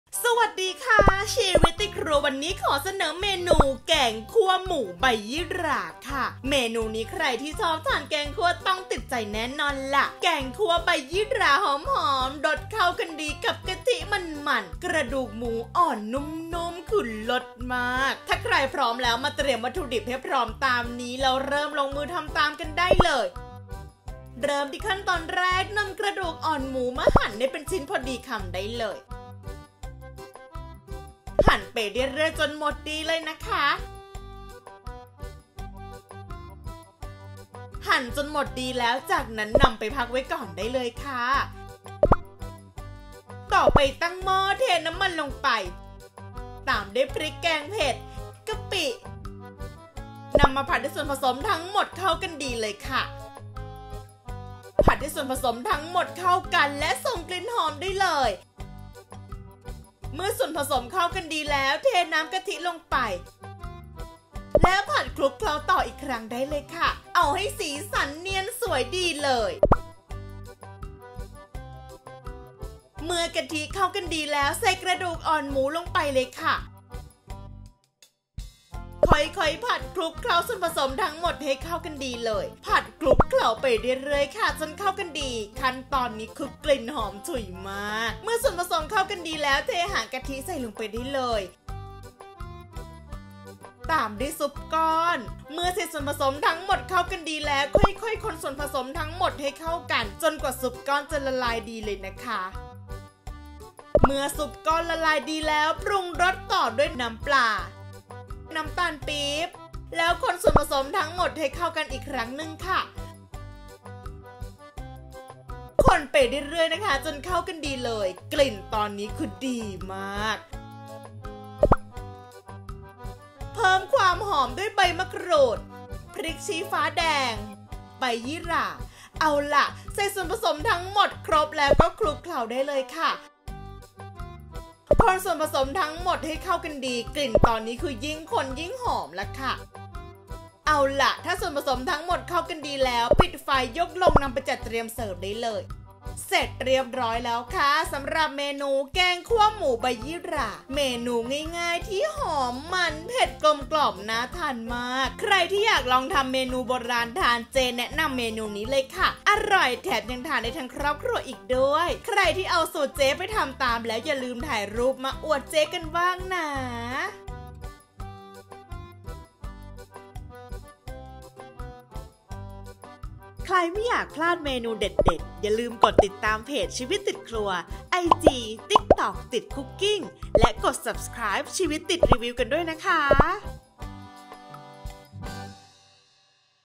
[แจกสูตร] แกงคั่วหมูใบยี่หร่า - ชีวิตติดครัว
สวัสดีค่าาชีวิตตดิครัววันนี้ขอเสอนเมนู แกงคั่วหมูบยี่หร่า เมนูนี้ใครที่ชอบทานแกงคั่วต้องติดใจแน่นอนละ แกงคั่วใบยี่หราหอมๆ รสเข้ากันดีกับกะทิมันๆ กระดูกหมูอ่อนนุ่มๆ คือเริ่ดมากก ทานกับข้าวสวยยิ่งดีกันไปใหญ่ ถ้าใครพร้อมแ้วมาเตรียมวัตถุดืบให้พรอ้มตามนี้แล้วมาลงมือทำกันได้เลยค่าา

พริกแกงเผ็ด   2  ช้อนโต๊ะ
กระดูกหมูอ่อน 700  กรัม
กะปิ   1  ช้อนโต๊ะ
ใบยี่หร่า  1  กำ
ซุปก้อน  1 ก้อน
พริกชี้ฟ้า  4-5  เม็ด